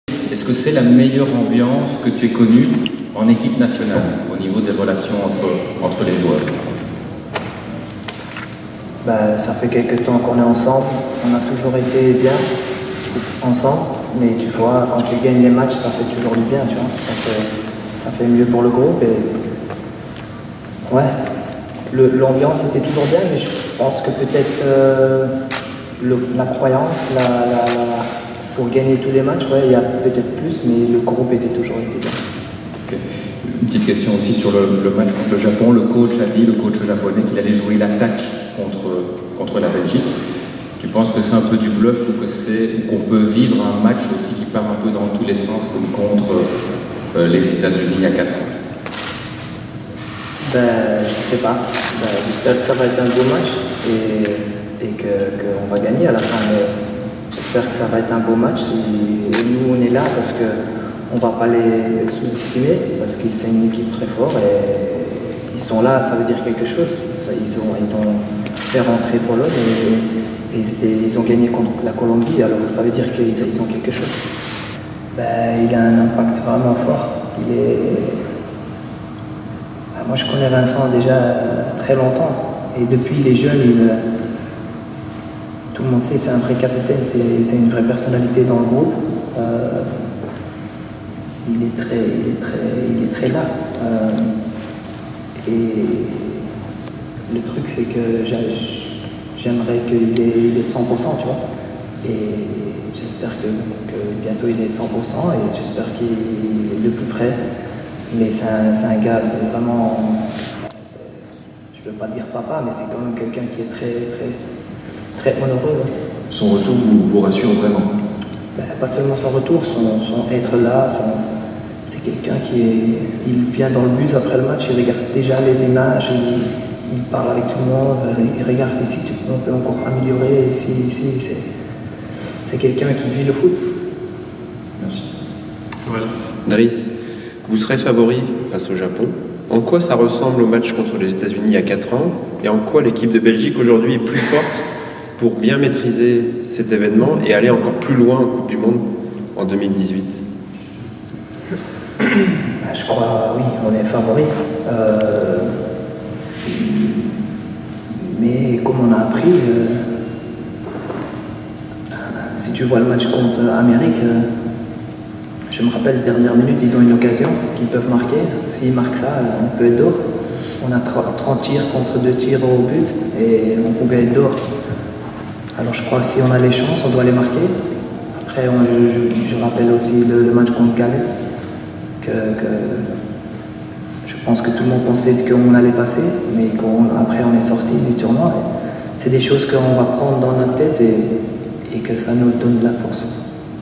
D. Mertens «J'espère que ça va être un beau match» - Foot - CM 2018 - BEL
L'attaquant des Diables Rouges Dries Mertens est revenu en conférence de presse sur la manière dont son équipe abordait son huitième de finale de Coupe du monde face au Japon (lundi, 20h).